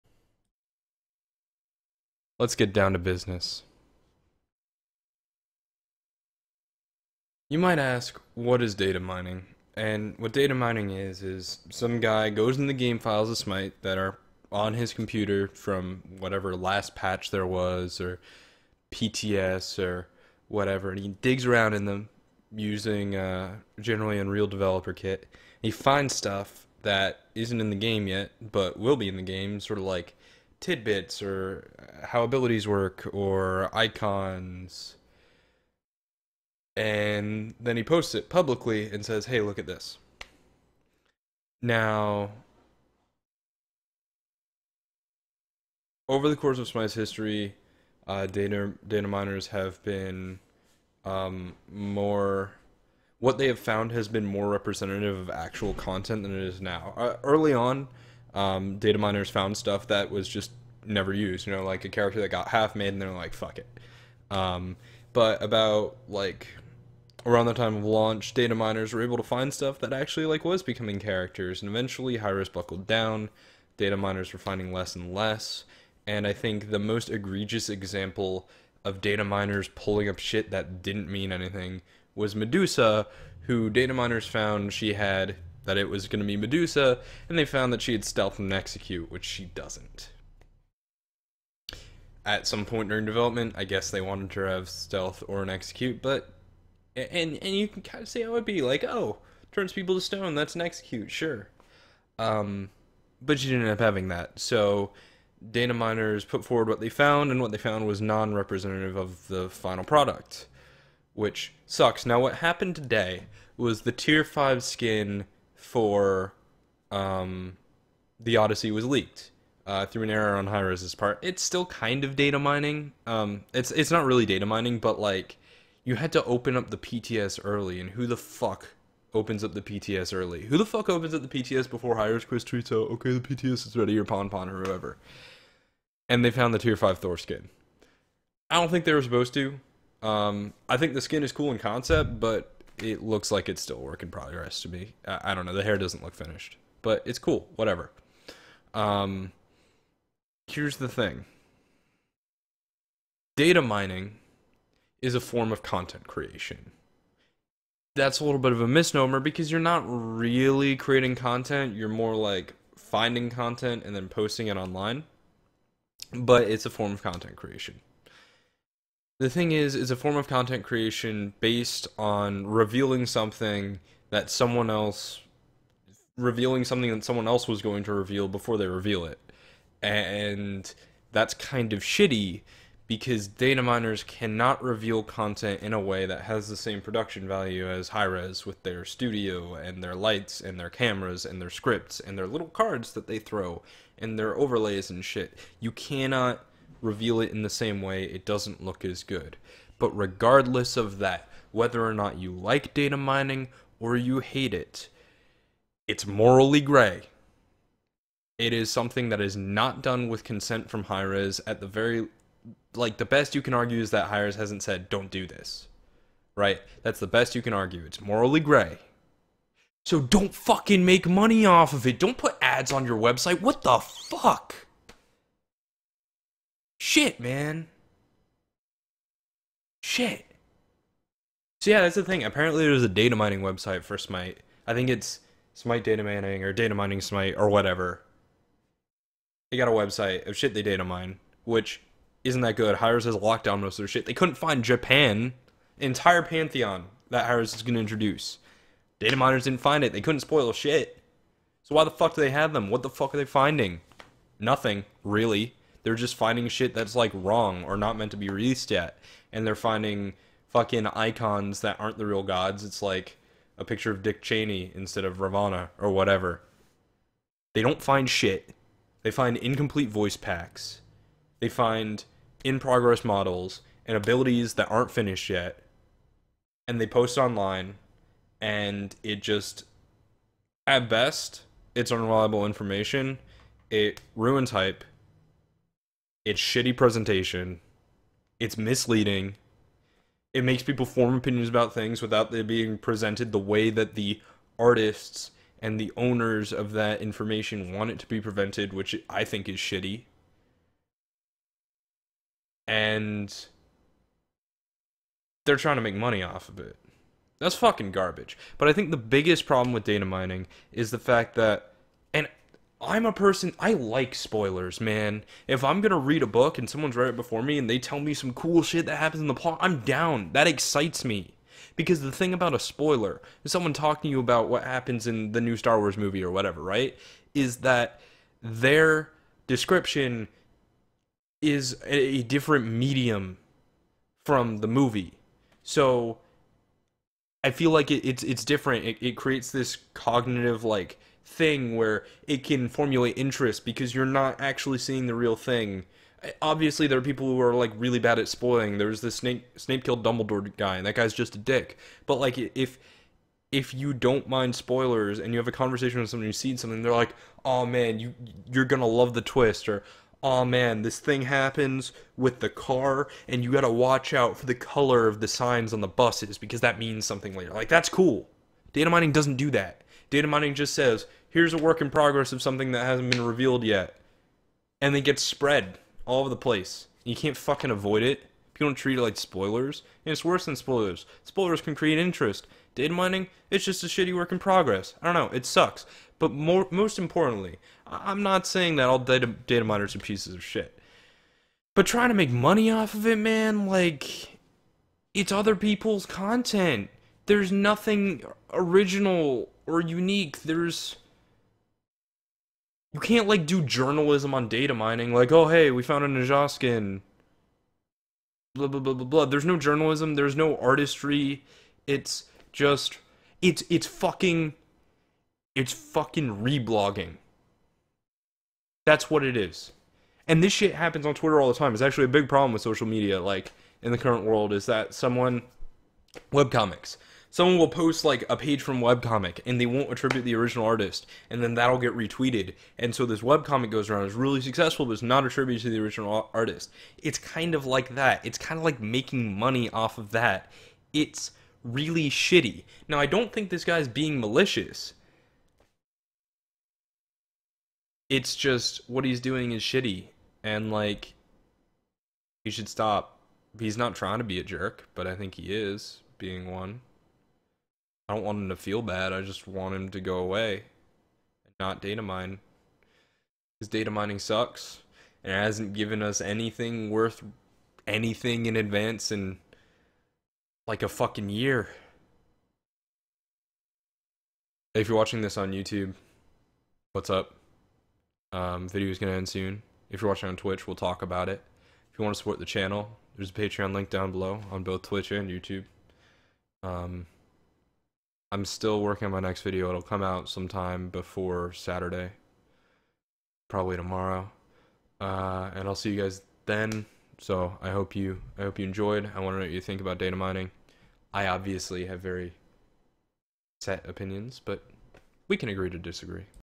Yeah, Really. Datamining (sucks!)
I think datamining sucks, let's talk about why.